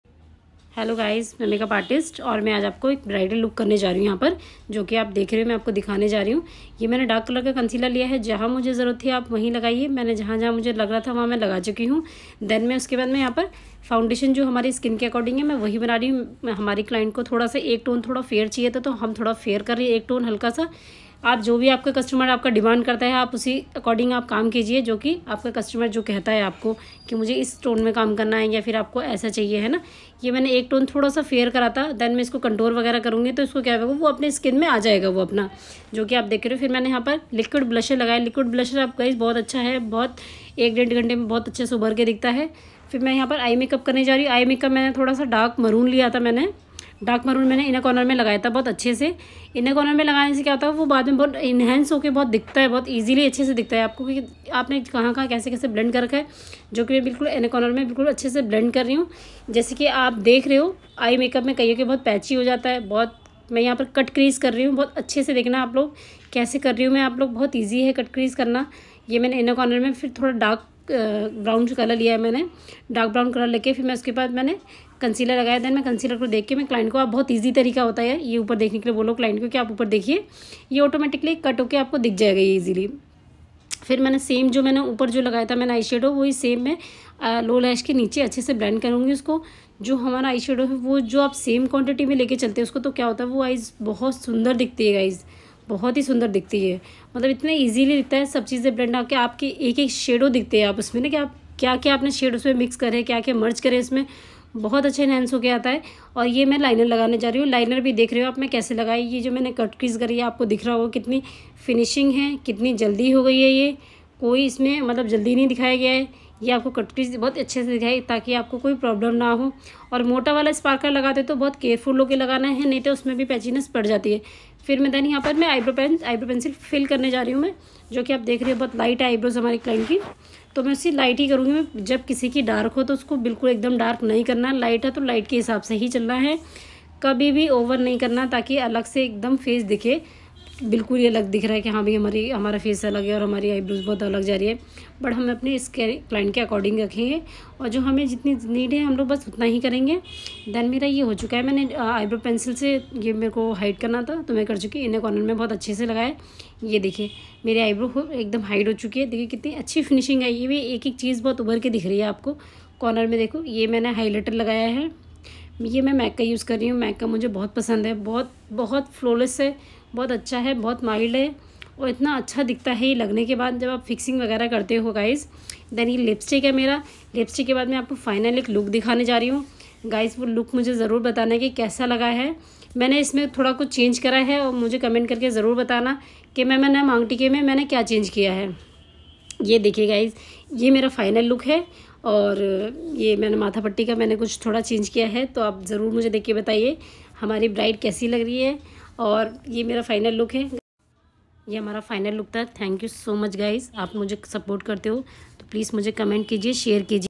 BRIDAL MAKEUP KESE KARE / BRIDAL MAKEUP STEP BY STEP / ￼￼ HOW TO DO BRIDAL MAKEUP
HLO DOSTO BRIDAL MAKEUP KESE KARE AP IS VIDIEO LO JARUR PURA DEKHIYE IS VIDEIO ME APKE SARE DAUT KHATAM HO JAENGE KU YE VIDEIO MENE UN LOGO KE LIYE BANYA JO BHUT JADA PARESHAN REHTE KI UNKA MALEUL KALA PAD JATA HE Wo log jarur is makeup ko dekhe

Ye mera insta link he